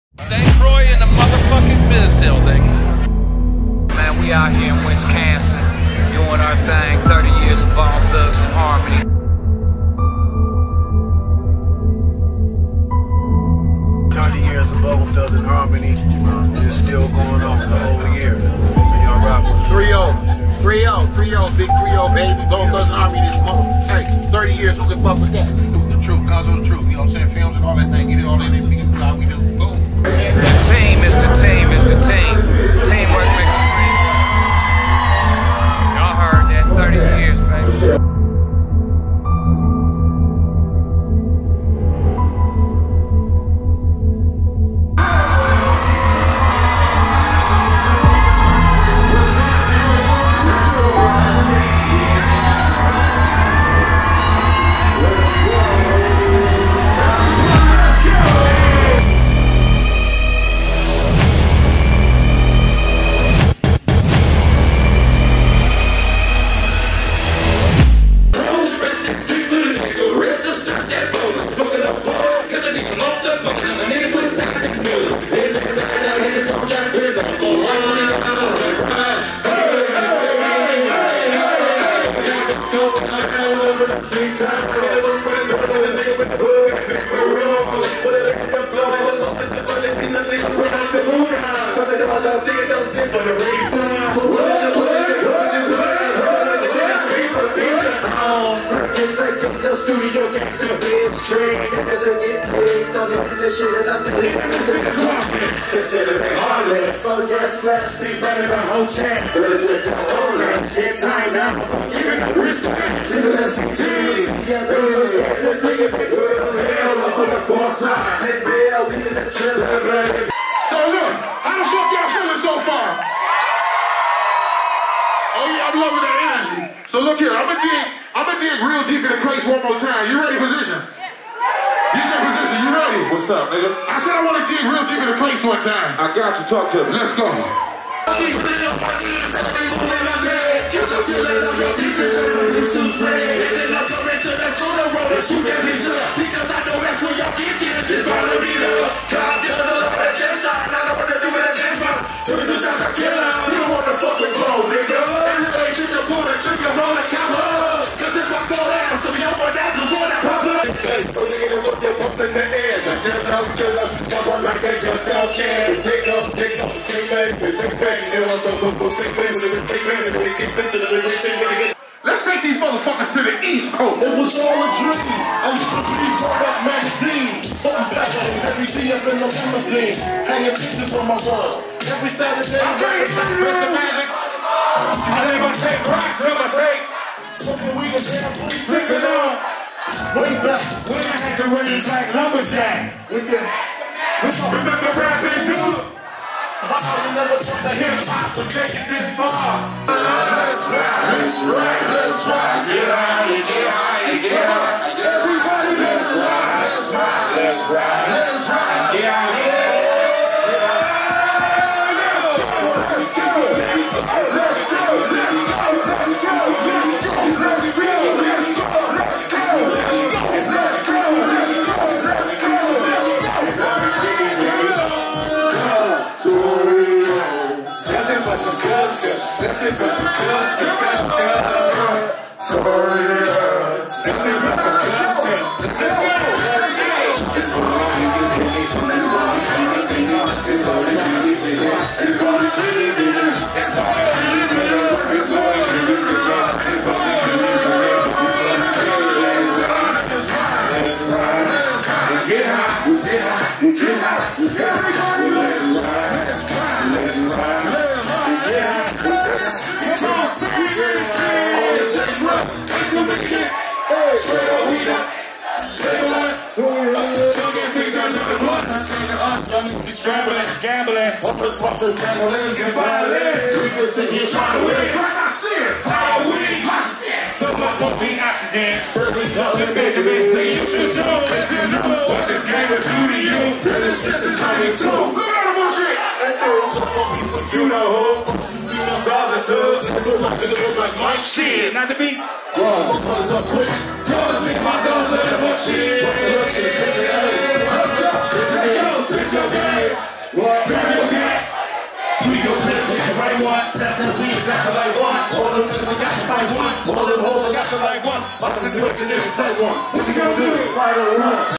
BONE THUGS N HARMONY 8-9-24
BTNH PERFORMS LIVE IN ST CROIX WISC. 2024